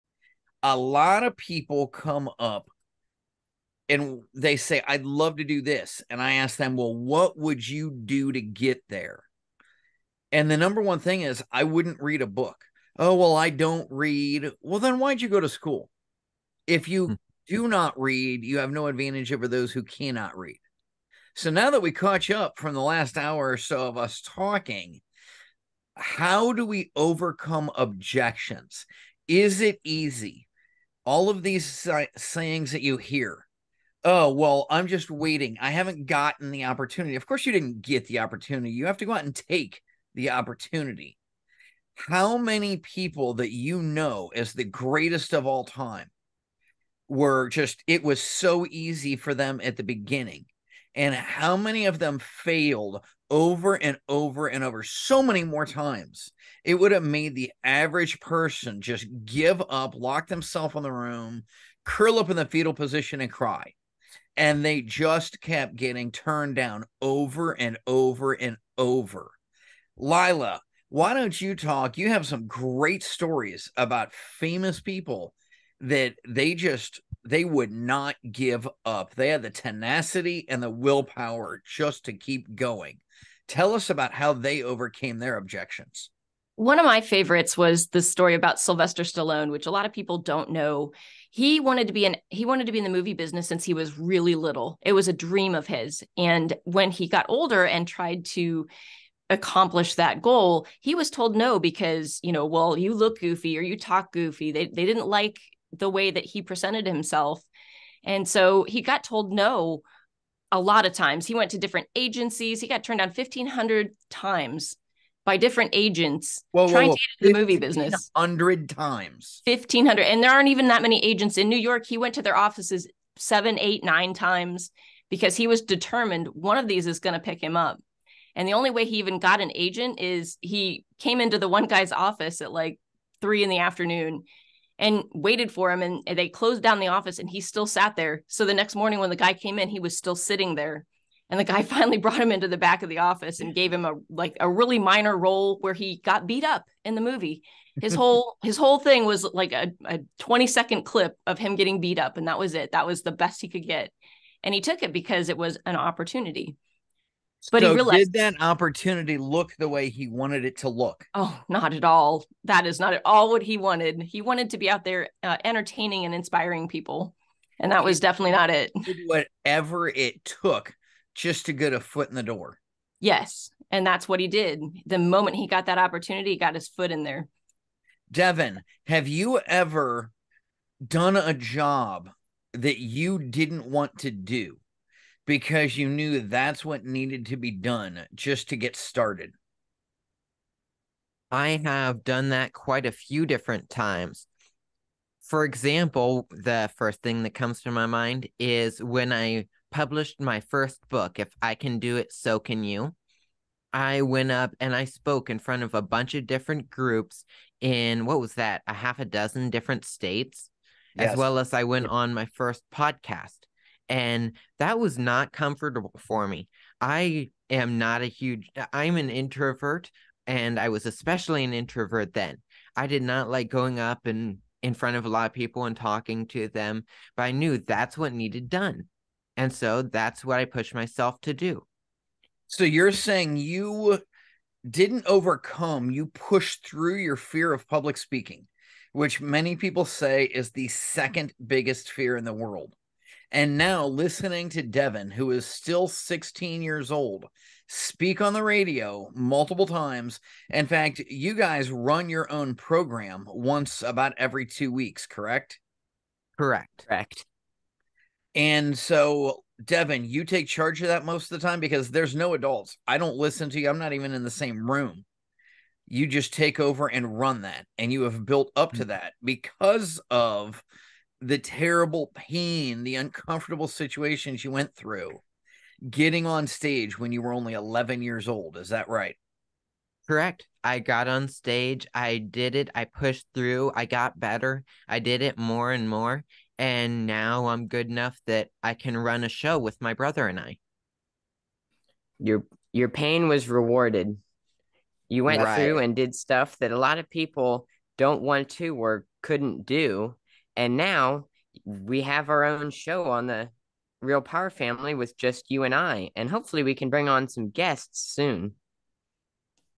Overcome your obstacles
💁🏼‍♂📣 In Episode 27 of the Real Power Family Radio Show, The Real Power Family talks about how to overcome obstacles in your life.💥

💁🎙 Listen to the full show on RealPowerFamily.com 👈